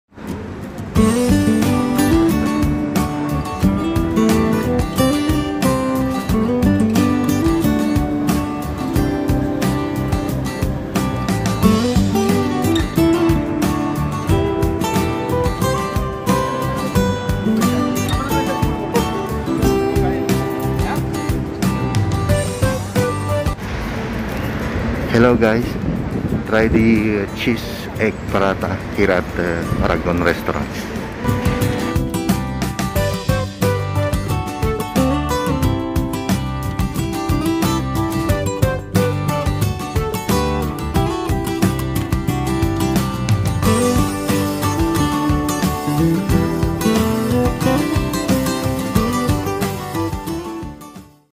WHITE PARAGON RESTAURANT | TRY THE CHEESE, EGG PARATHA | MEI YT
WHITE PARAGON RESTAURANT | TRY THE CHEESE, EGG PARATHA | CENTRAL DISTRICT, TOWN CENTER, AL AIN CITY | MEI YT